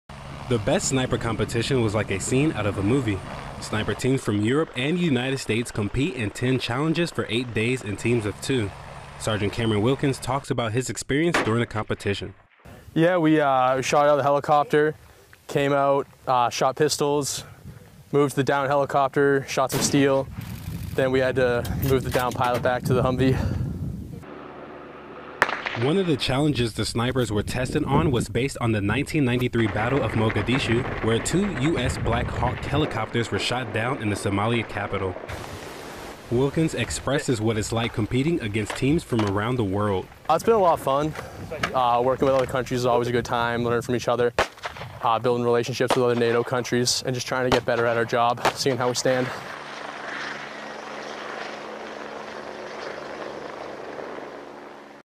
European Best Sniper Competition
United States and European competitors snipe their way to see who is the best. Snipers from 18 countries visited the Joint Multinational Readiness Center to compete in the European Best Sniper Competition.

HOHENFELS, BY, GERMANY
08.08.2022

This video was filmed on August 8, 2022
Video by SGT Michael Williams, AFN Bavaria
Produced by SGT Michael Williams

Lower third information:

@00:12
SGT Cameron Wilkins
Competitor

Film Credits: Video by Pfc. Kaleb Kennedy and Sgt. Michael Williams
AFN Bavaria